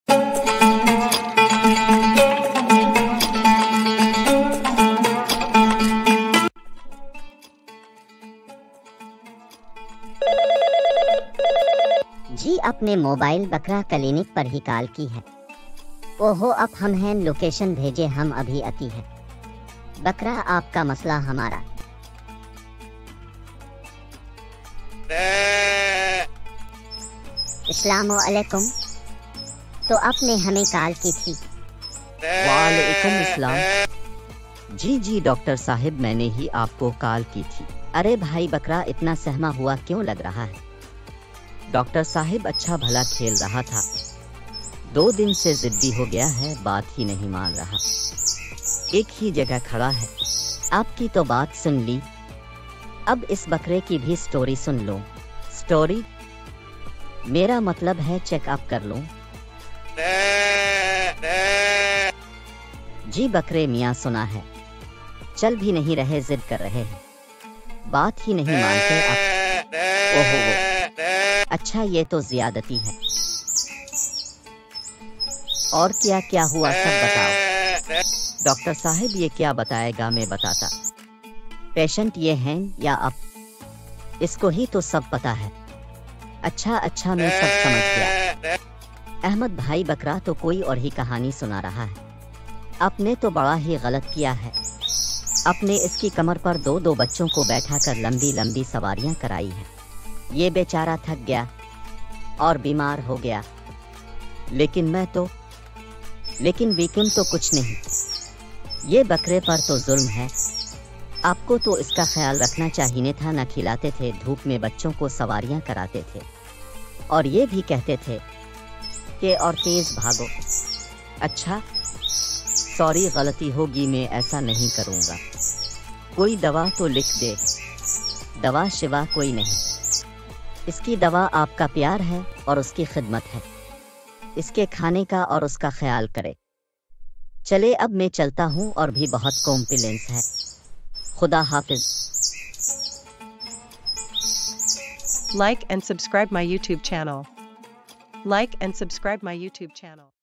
Mobile Bakra Clinic | موبائل بکرا کلینک Bakra Eid 2023 |Kids Cartoon In Urdu
On the occasion of Eid ul Adha 20213, Our Channel has released Public Service Message for Kids "Mobile Doctor Bakra Clinic" , Like and Share this beautiful Kids Bakra Eid 2023 Story.